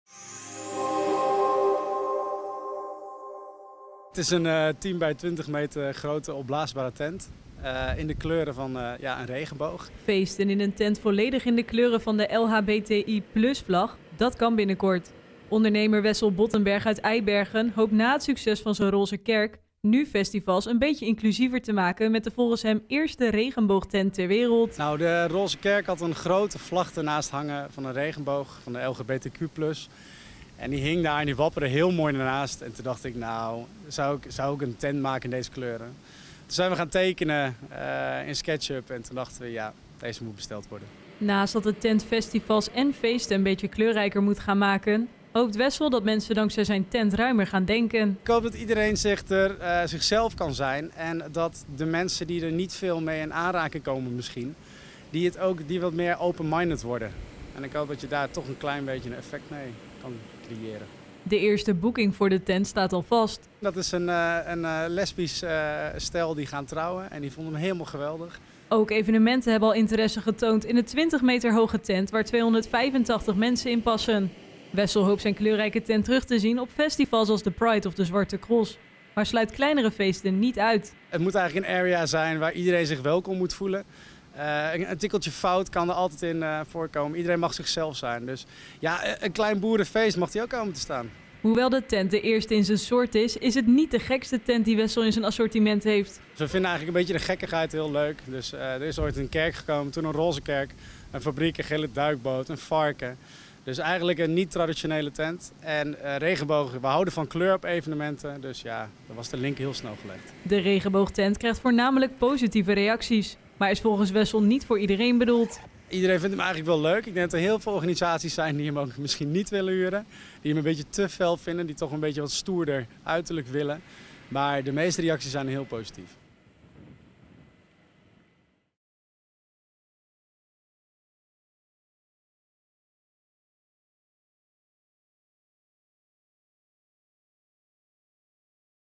Eibergenaar wil met regenboogtent festivals en feesten inclusiever maken
Feesten volledig in de kleuren van de LHBTI+-vlag, dat kan binnenkort. Ondernemer Wessel Bottenberg uit Eibergen hoopt na het succes van zijn roze kerk, nu festivals een beetje inclusiever te maken met de volgens hem eerste regenboogtent ter wereld.